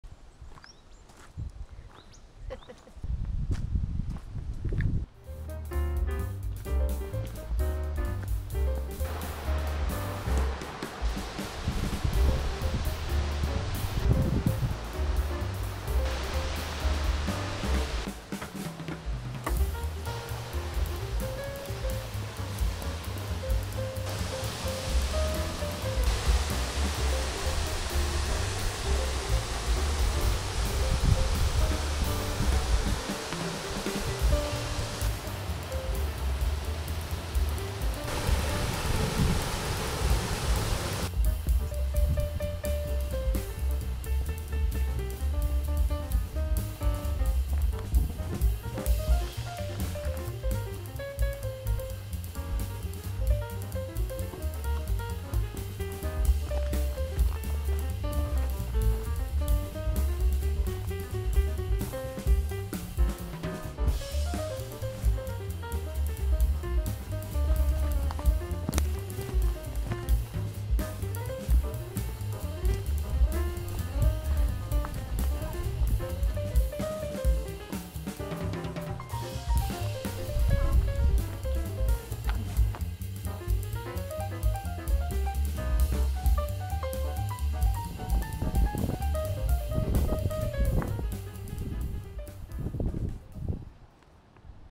Carrington Falls NSW (Budderoo National Park)
With its cascades, waterholes and rockpools, some regard Carrington Falls, where the Kangaroo River plunges over 50m to the gully below, as the most beautiful on the South Coast.